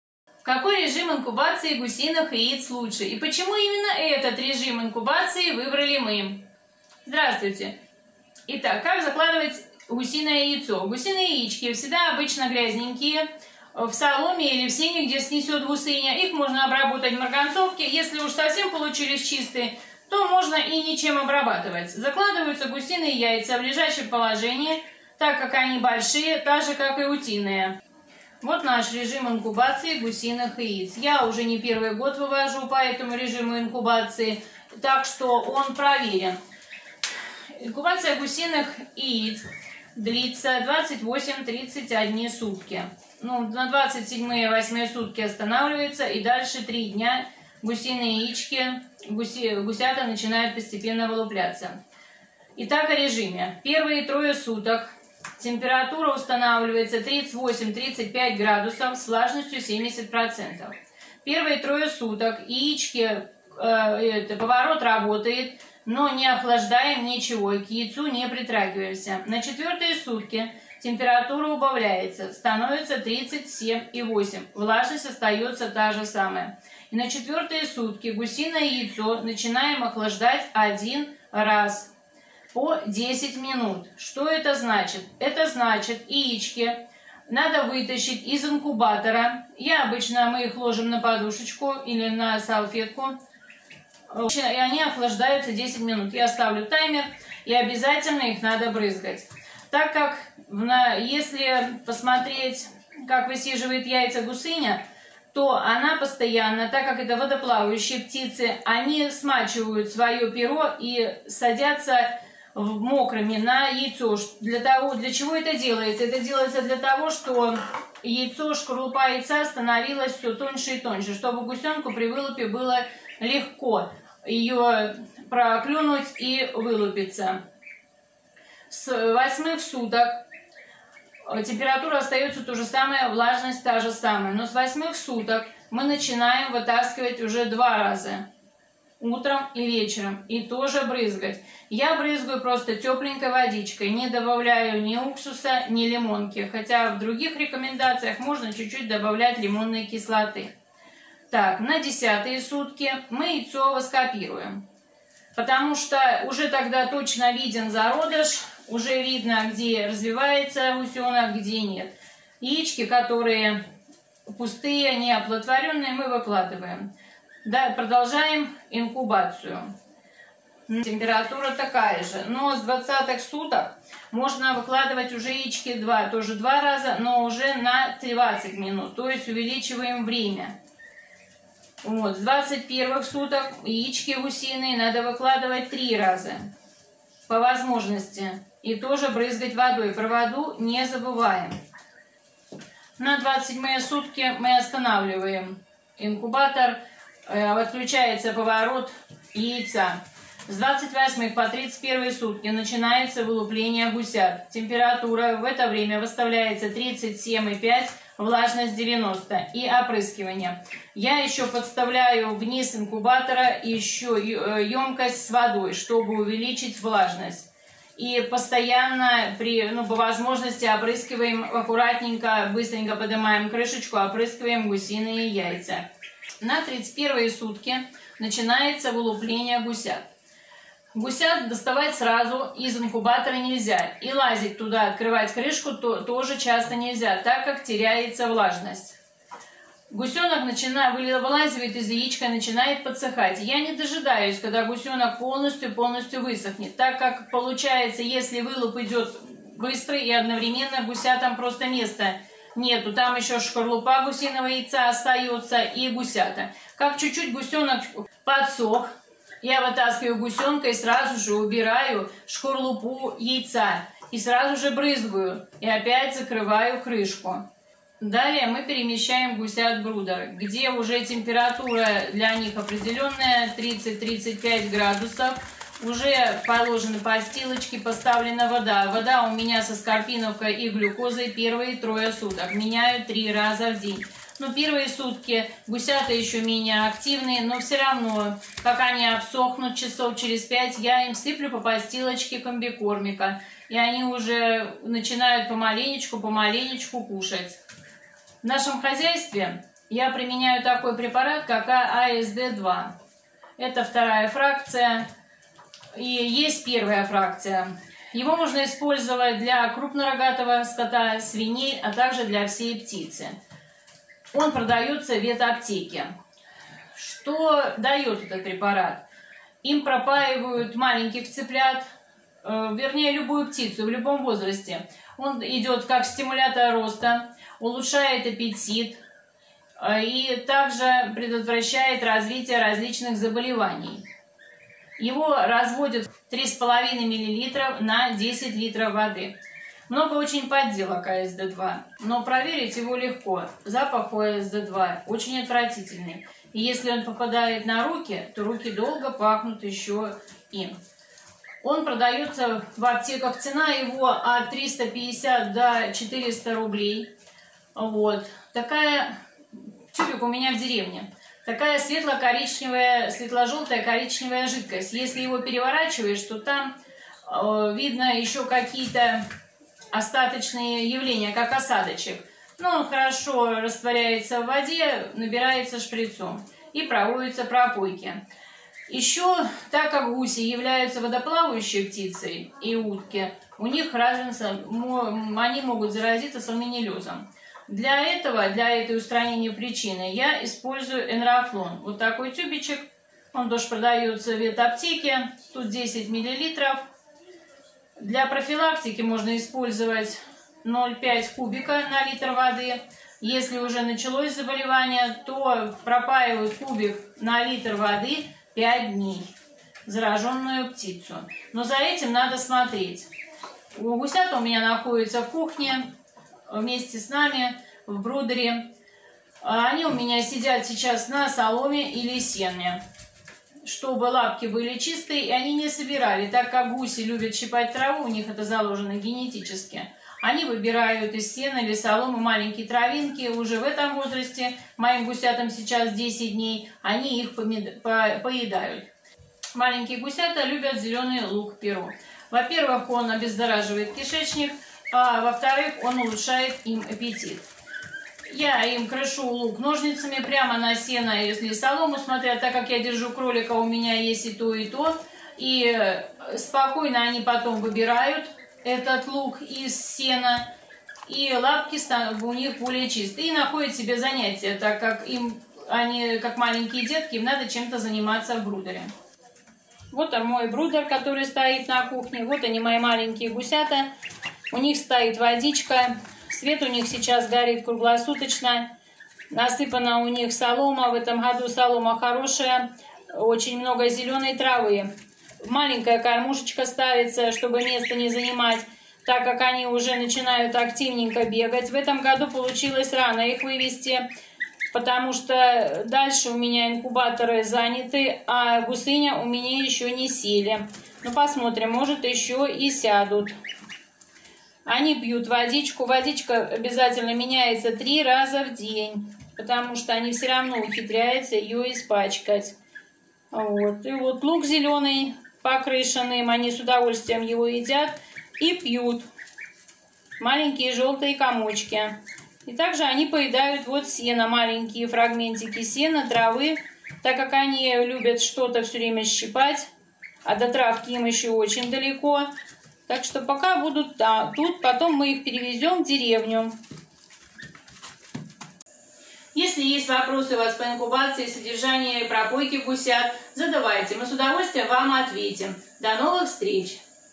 Какой режим инкубации лучше для гусиных яиц
В данном видео мы расскажем  о нашем режиме инкубации гусиного яйца - какая должна быть температура. влажность на протяжении  всей инкубации. чем пропаивать и кормить в первые дни после вылупленния гусят.